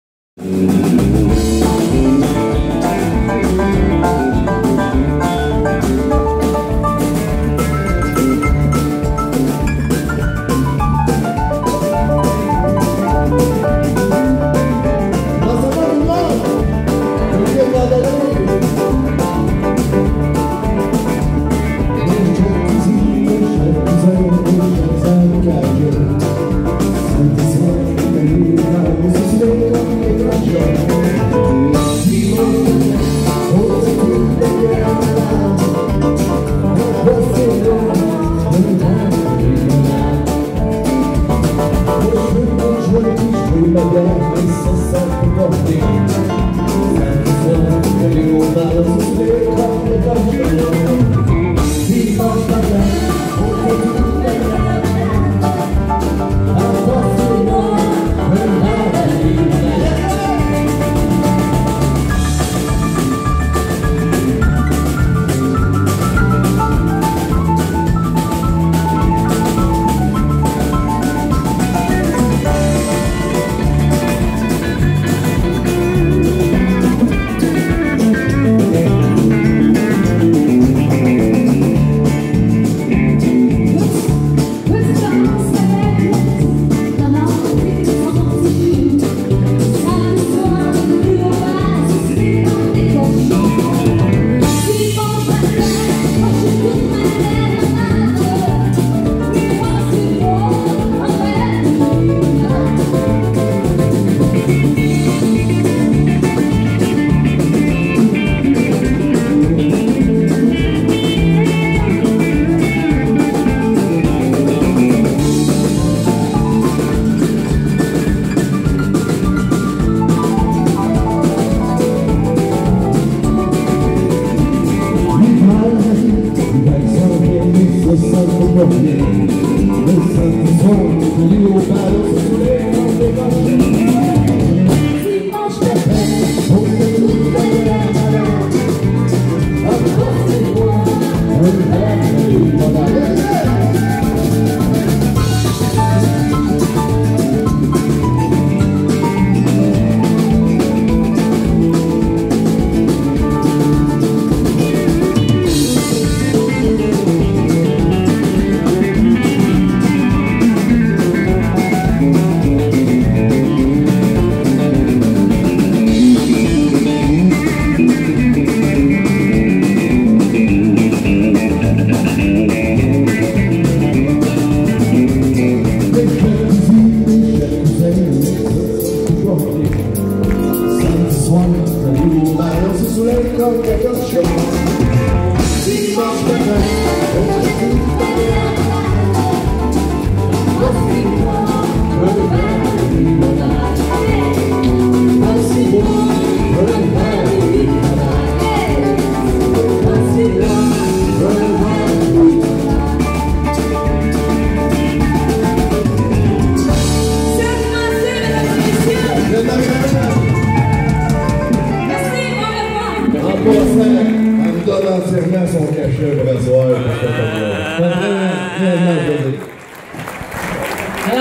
Linda Gravel et Serge Massé au festival de Roland Pelletier à St-Paulin 2016
Linda Gravel et Serge Massé  au festival de Roland Pelletier à St-Paulin 2016 Ne pas oublier de vous abonnez à ma chaine YouTube gratuite, en cliquant sur ma petite photo en bas à droite, merci d'avance ...